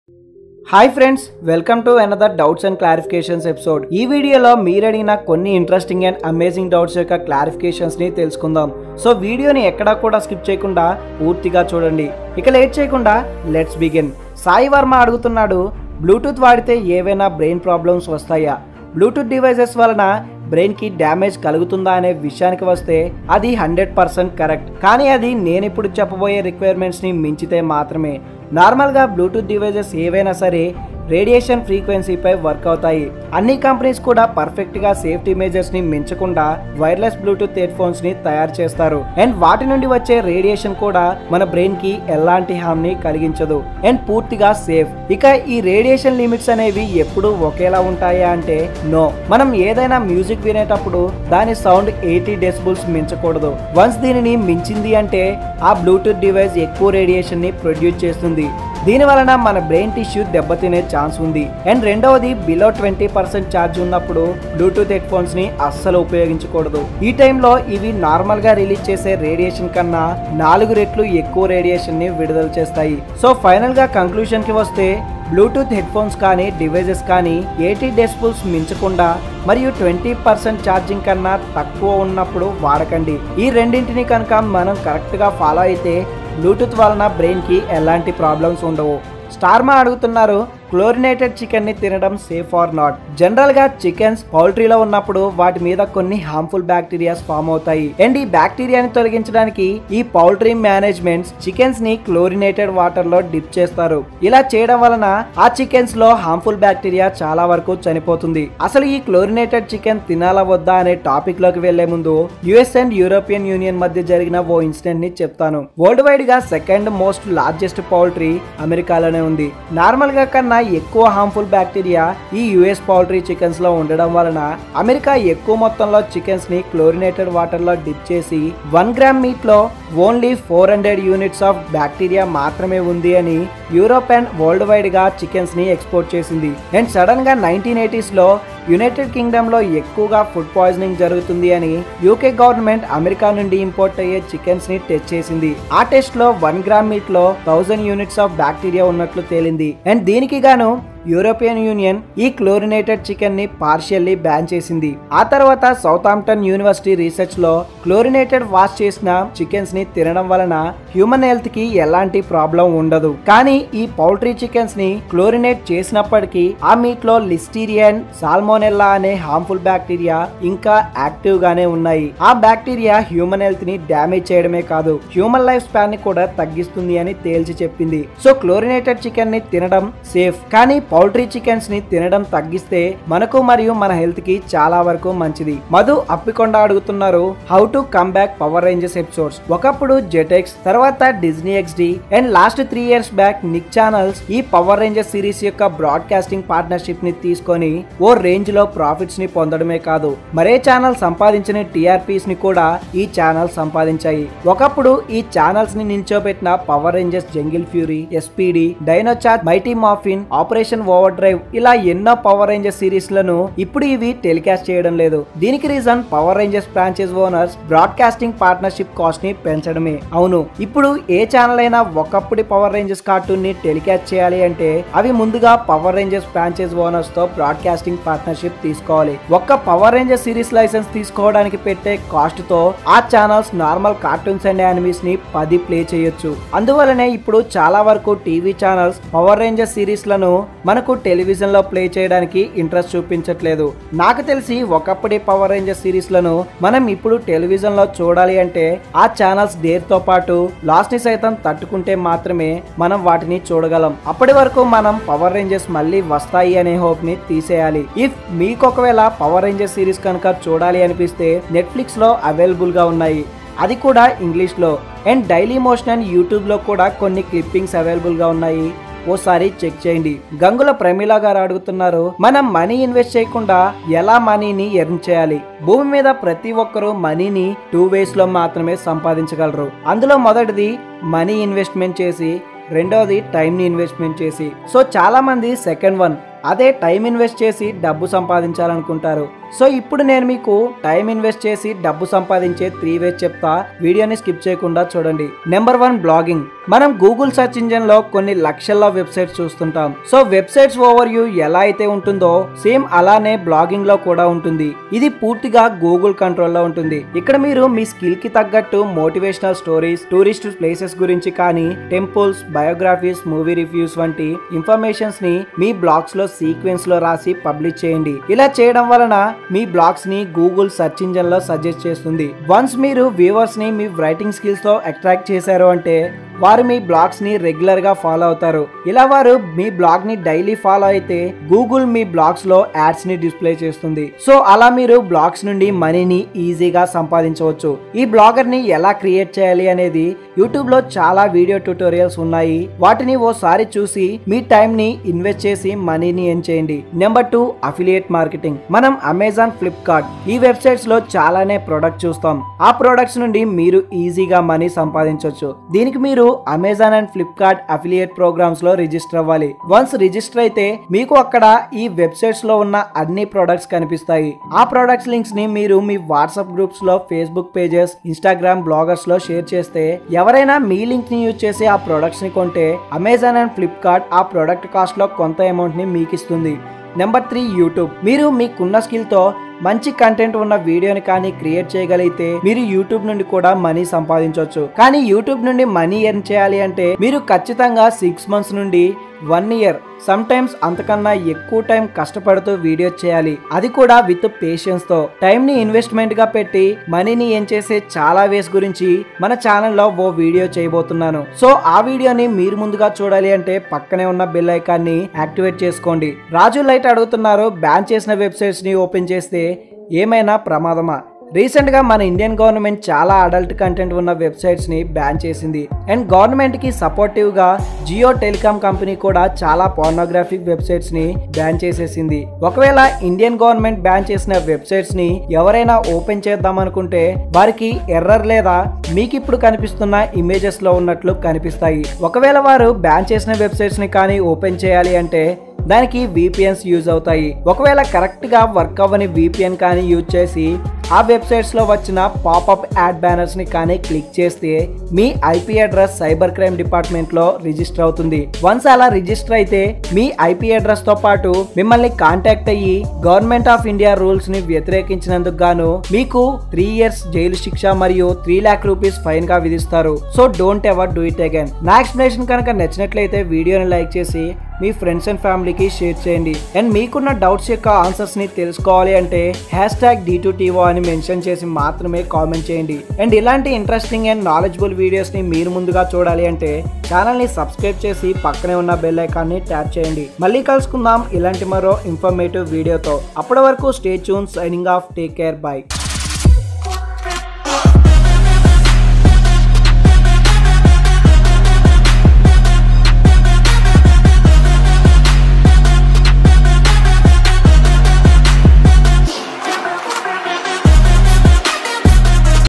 MOST AMAZING DOUBTS & CLARIFICATIONS | HOW TO EARN MONEY WITHOUT INVESTMENT | BLOGGING
Welcome to another amazing doubts and clarifications episode 09. also called

Here are the questions i picked to clarify

1.Is there any problems to brain by using blutooth device?
2.Does chlorinated chicken is safe to eat?
3.Does powe rangers comeback again?
4.how to earn money without investment?
in this question i mainly discussed about

5.Is there any problem if we try to open blocked websites?